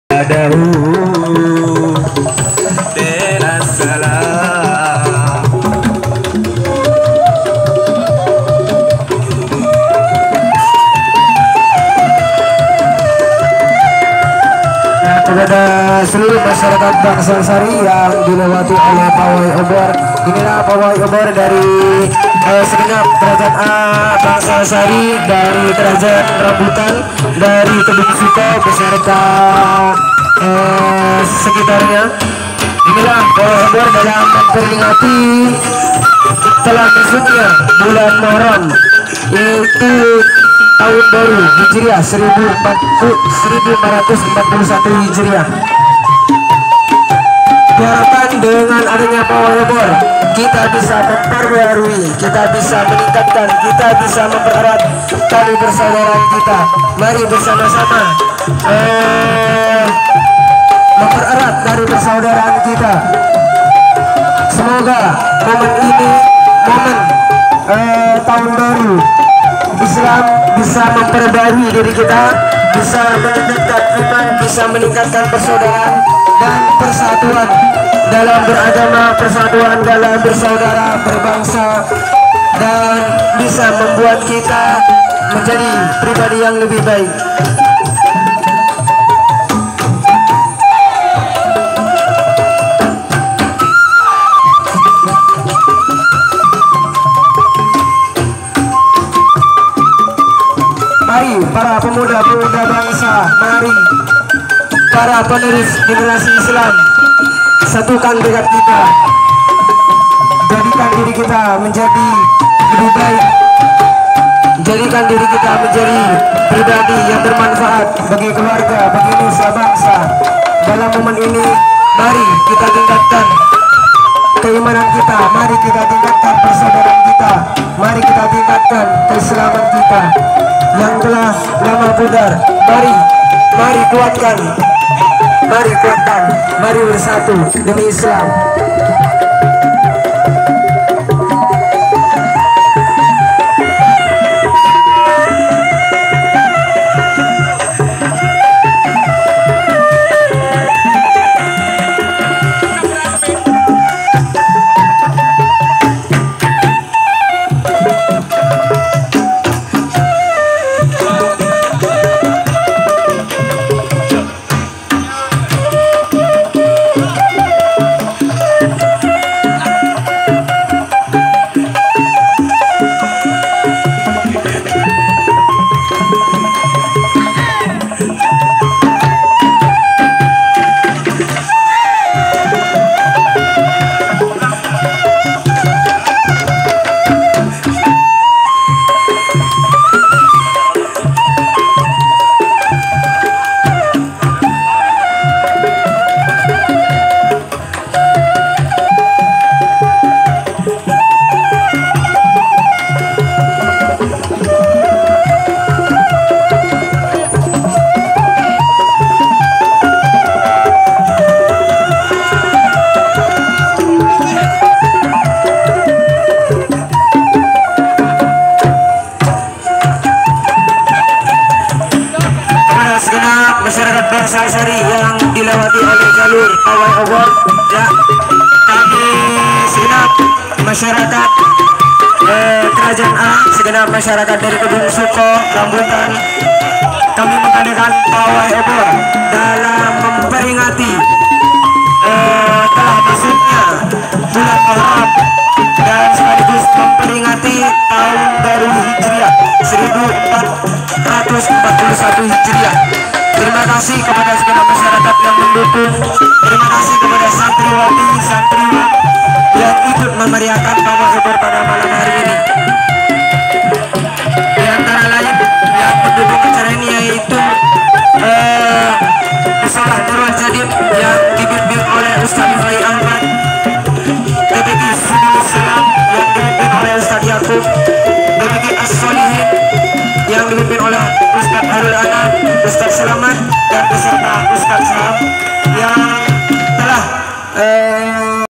Meriah Pawai Obor Masal Kecamatan Bangsalsari Jember Dalam Memperingati Tahun Baru Islam 1441H
Pawai di adakan di kecamatan bangsalsari kab.jember jatim , di ikuti dg 3 desa yaitu desa rambutan kedungsuko krajanA dan sekitarnya